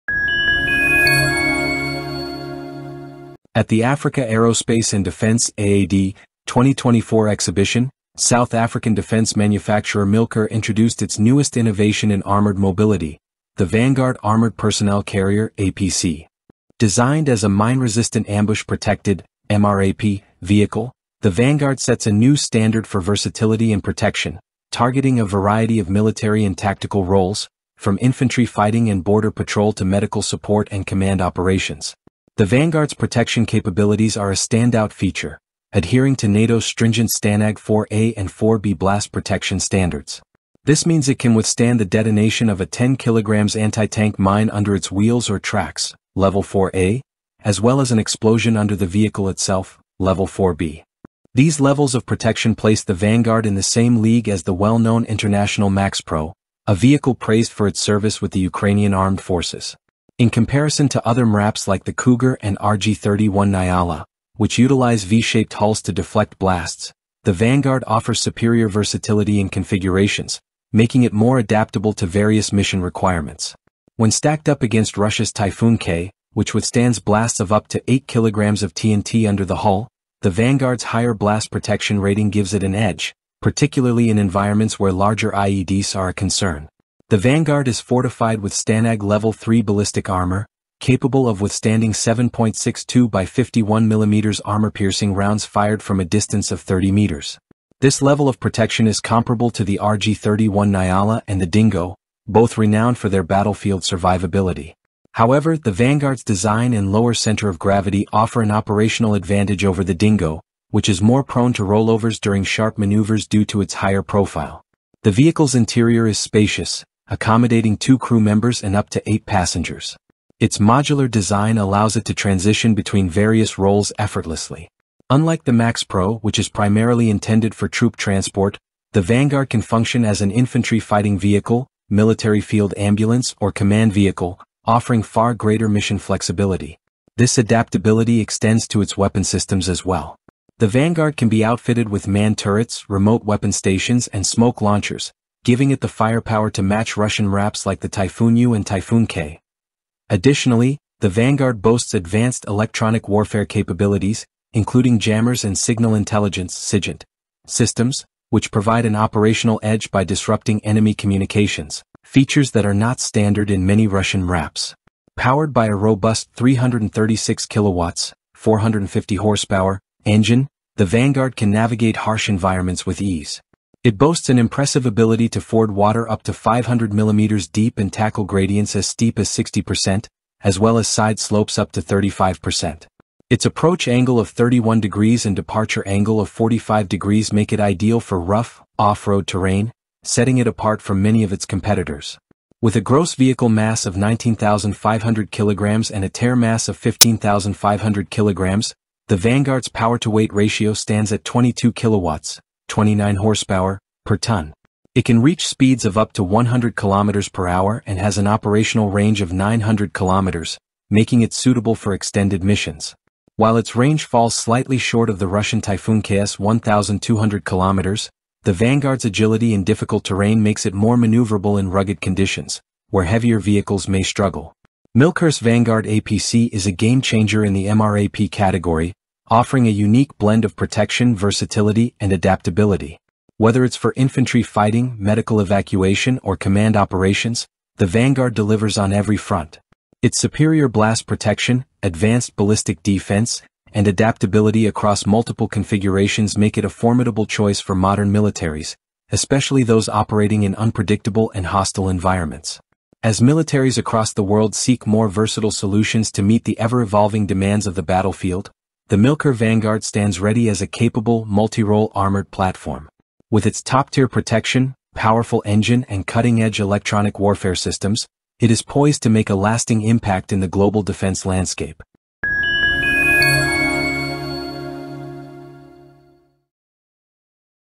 Milkor's new Vanguard APC challenges conventional MRAPs with superior capabilities
At the Africa Aerospace and Defence (AAD) 2024 exhibition, South African defense manufacturer Milkor introduced its newest innovation in armored mobility: the Vanguard Armoured Personnel Carrier (APC). Designed as a Mine-Resistant Ambush Protected (MRAP) vehicle, the Vanguard sets a new standard for versatility and protection, targeting a variety of military and tactical roles, from infantry fighting and border patrol to medical support and command operations.